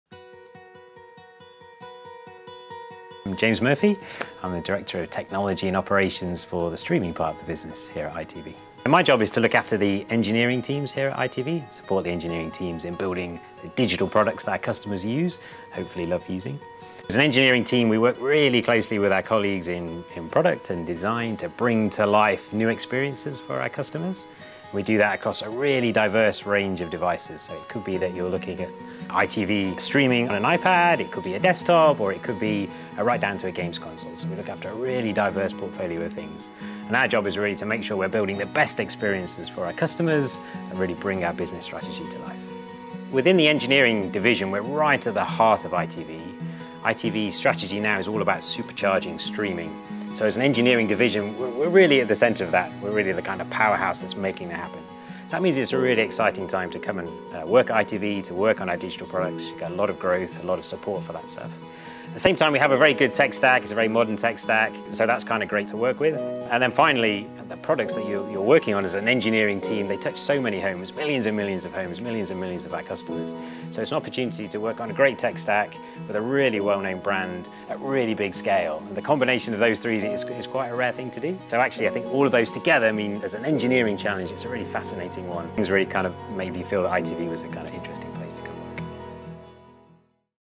Director of Technology - James Murphy
Heading up the streaming side of our business, James makes sure that our engineering teams have everything they need to bring fantastic experiences to life for our customers. From tablets to game consoles, it’s a big operation, and engineering is really at the heart of that. It’s an exciting time to get involved.

Head to itvjobs.com to find out more.